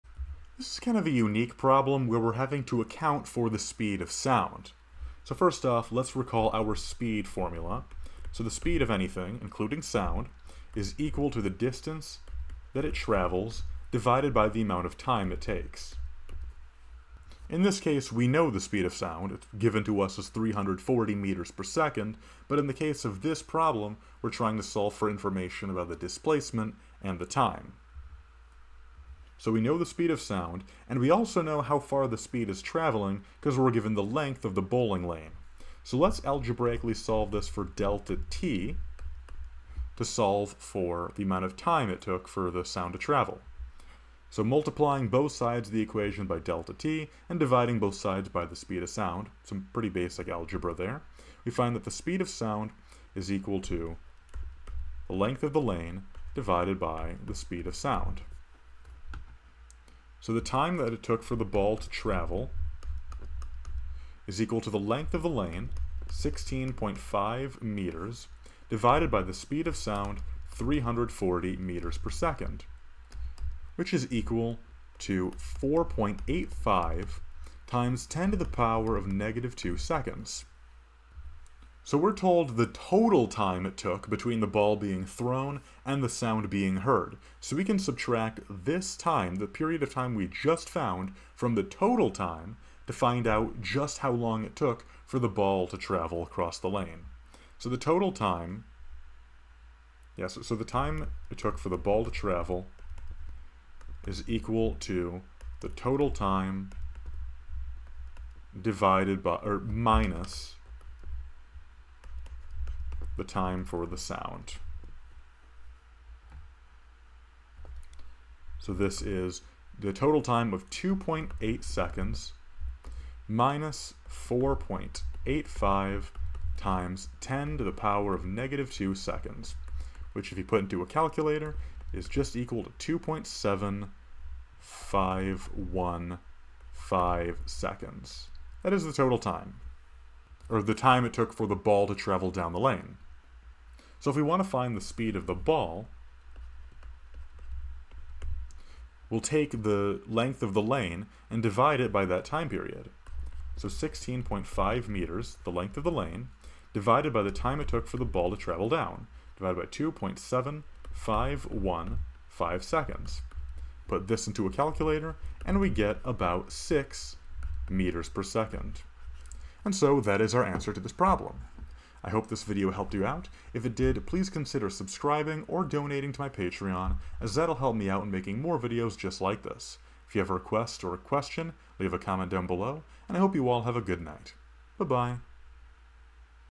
A bowling ball traveling with constant speed hits the pins at the end of a bowling lane 16.5 m long.
A bowling ball traveling with constant speed hits the pins at the end of a bowling lane 16.5 m long. The bowler hears the sound of the ball hitting the pins 2.80 s after the ball is released from his hands. What is the speed of the ball, assuming the speed of sound is 340 m/s?